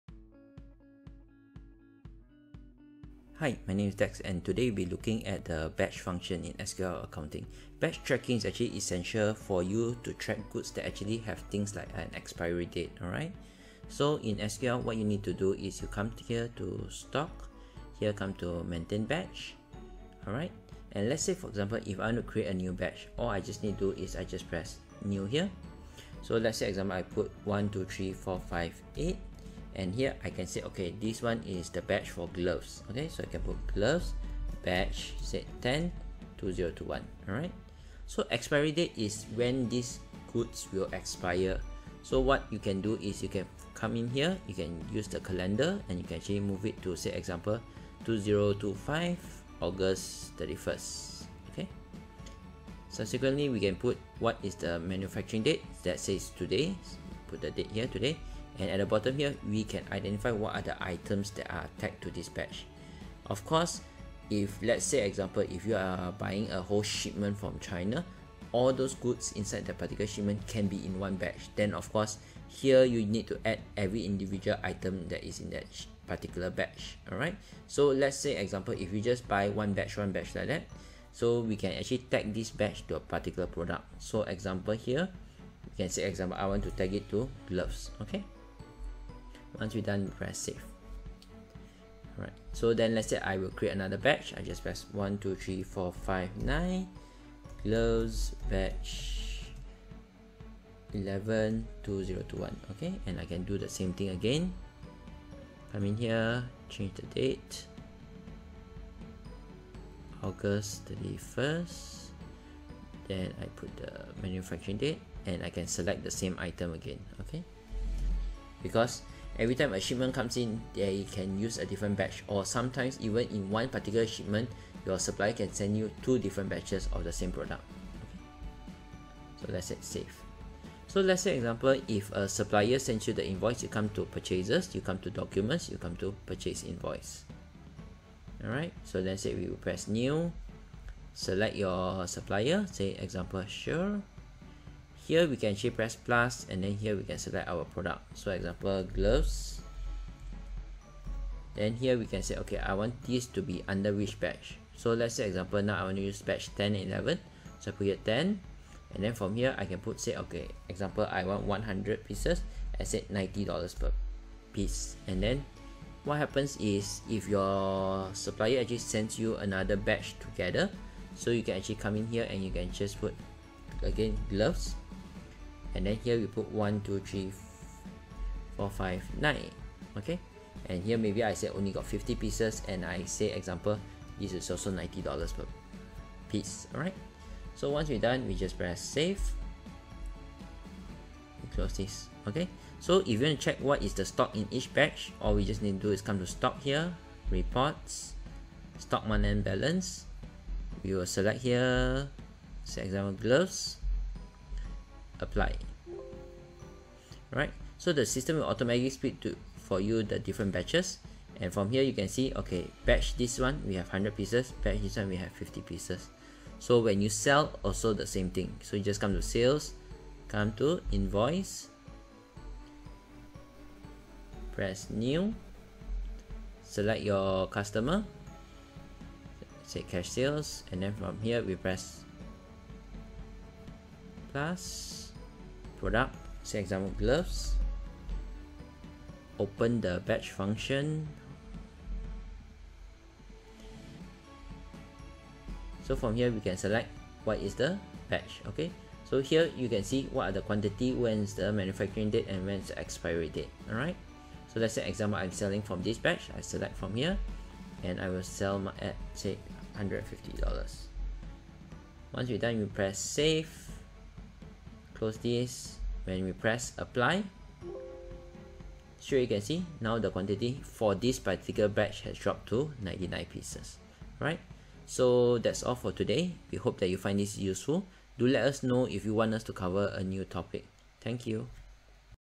SQL Accounting Tutorial - Batch Module
Need to manage batches for your stock?

Watch now and see how simple it is to do it in SQL Accounting.